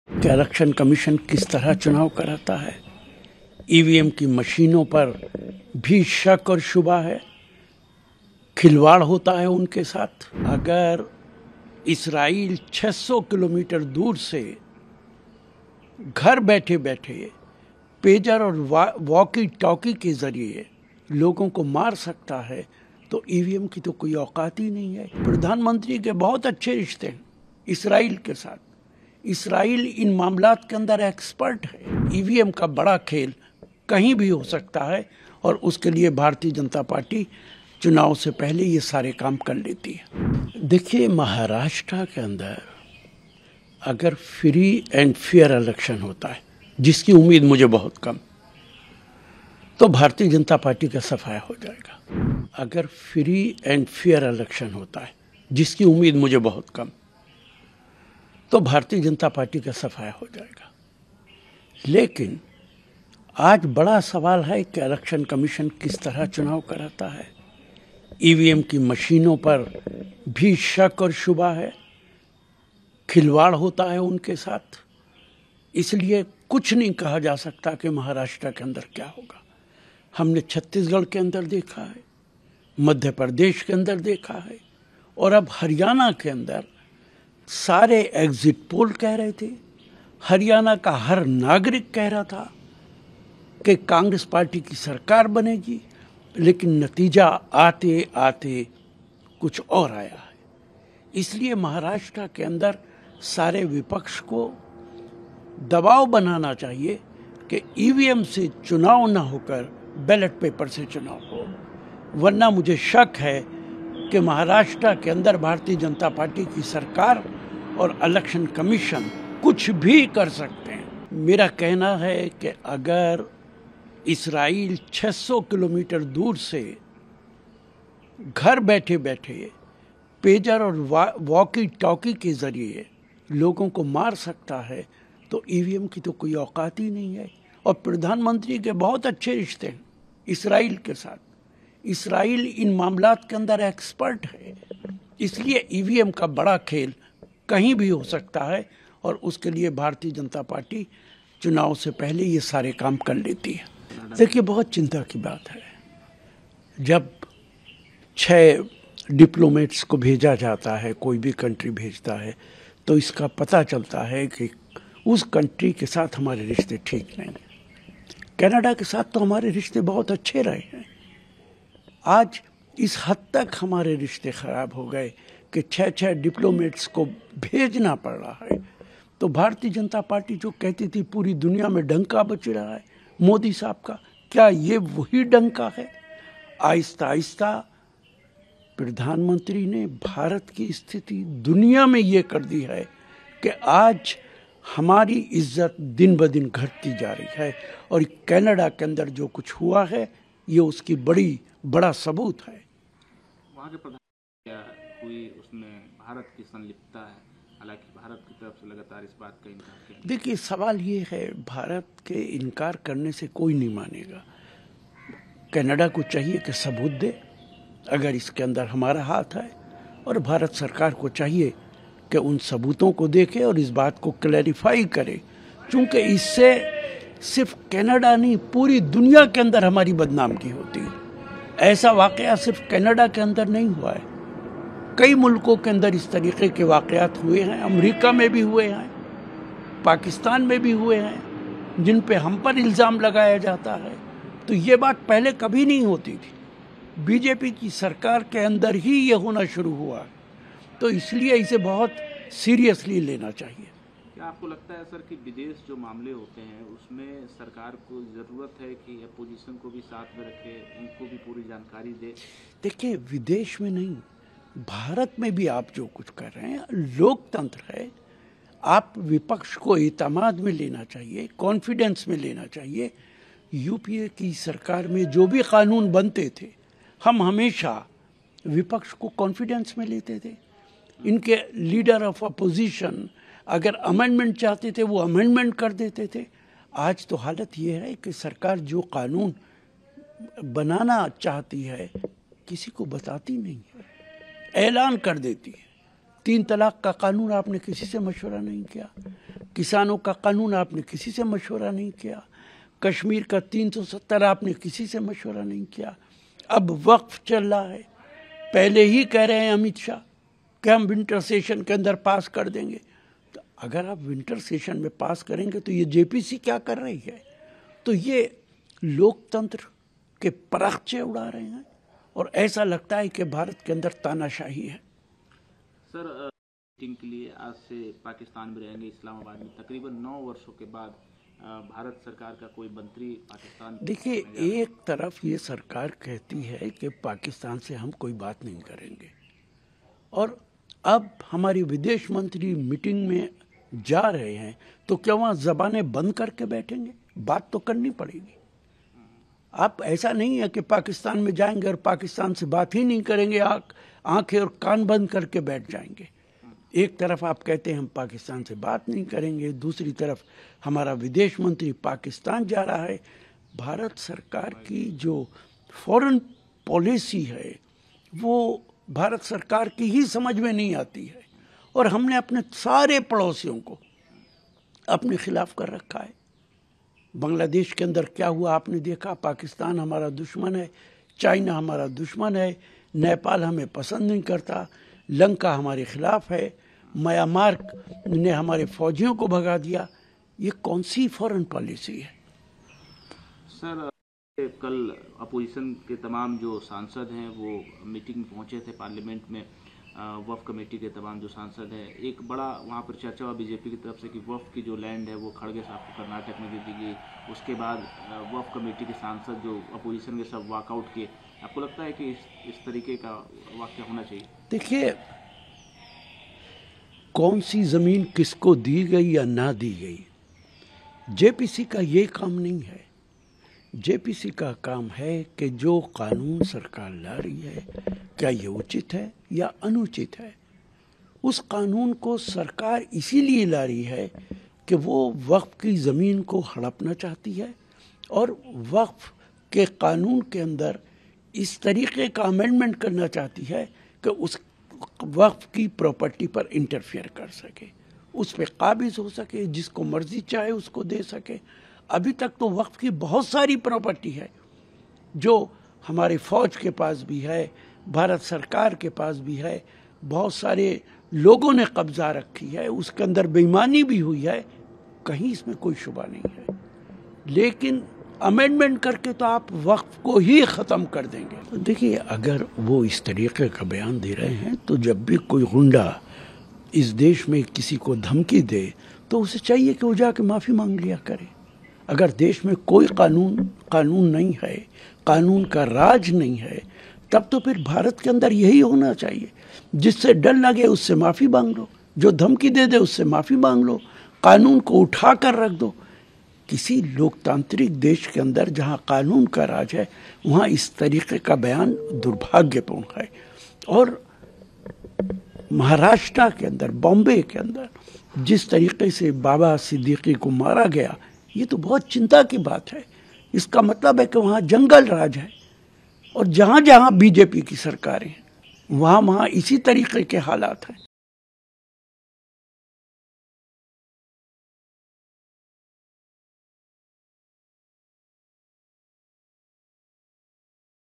Congress’ Rashid Alvi’s bizarre EVM claim; Compares with Hezbollah pager explosions
Ahead of the announcement of dates for the Maharashtra Assembly Elections, Congress’ Rashid Alvi made a bizarre EVM claim.
He said, "...In Maharashtra, the opposition should put pressure to insist on voting by paper ballot and not EVMs. Otherwise, in Maharashtra, BJP govt and EC can do anything. If Israel can kill people by use of pagers and walkie-talkies, then where does EVM stand? The PM has very good relations with Israel. Israel is an expert in such things. EVM ka bada khel kahin bhi ho sakta hai aur uske liye BJP chunav se pehle yeh sab khel kar leti hai."